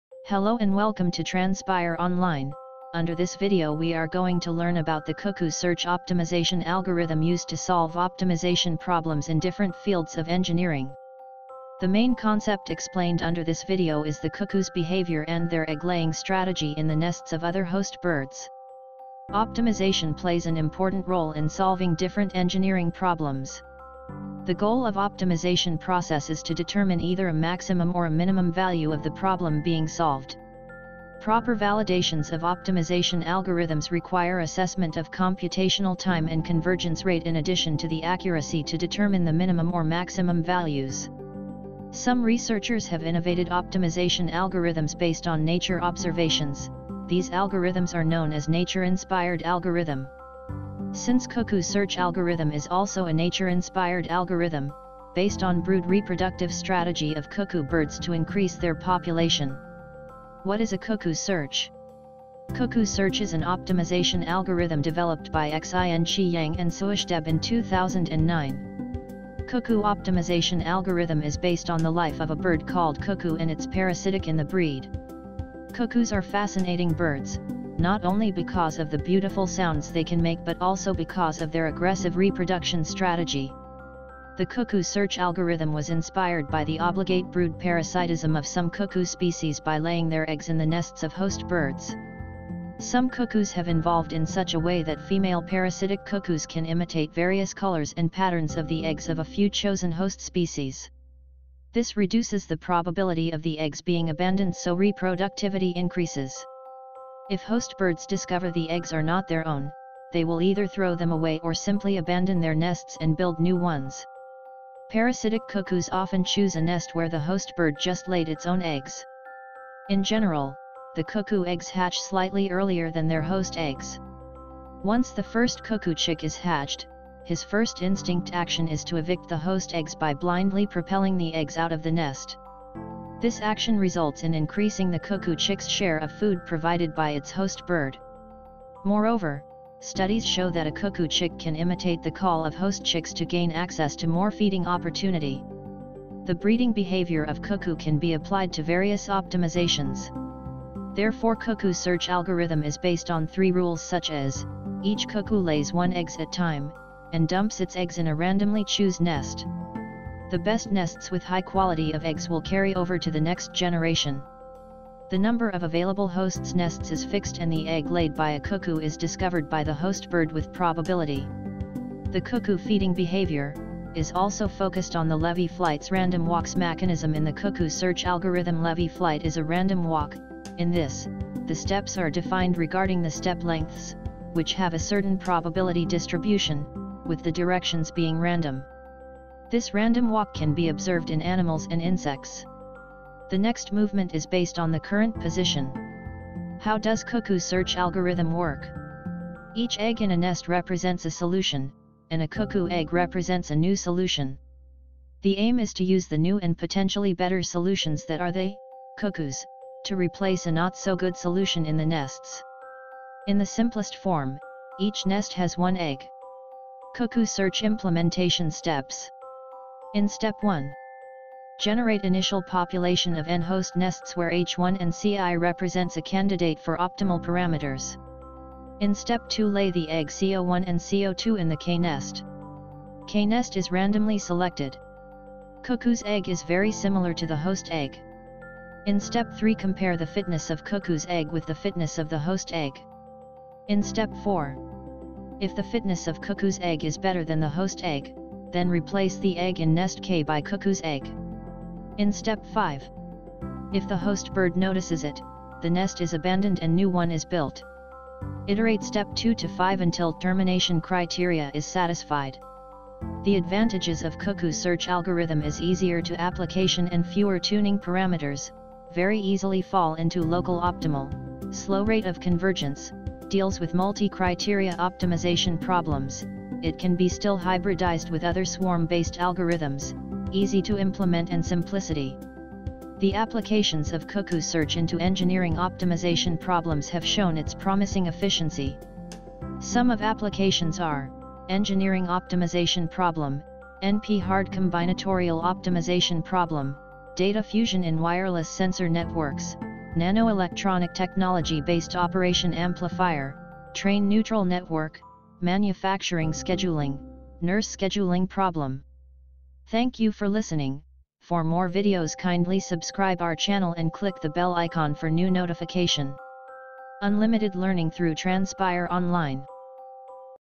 Decoderz #20 |26th October 2020| Cuckoo Search Optimization Algorithm -AI Tutorial
Cuckoo Search Optimization Algorithm
In this video, we will be discussing about  Cuckoo search is an optimization algorithm developed by Xin-she Yang and Suash Deb in 2009. It was inspired by the obligate brood parasitism of some cuckoo species by laying their eggs in the nests of other host birds (of other species). Some host birds can engage direct conflict with the intruding cuckoos. For example, if a host bird discovers the eggs are not their own,  it will either throw these alien eggs away or simply abandon its nest and build a new nest elsewhere.Some cuckoo species such as the New World brood-parasitic Tapera have evolved in such a way that female parasitic cuckoos are often very specialized in the mimicry in colors and pattern of the eggs of a few chosen host species Cuckoo search idealized such breeding behavior, and thus can be applied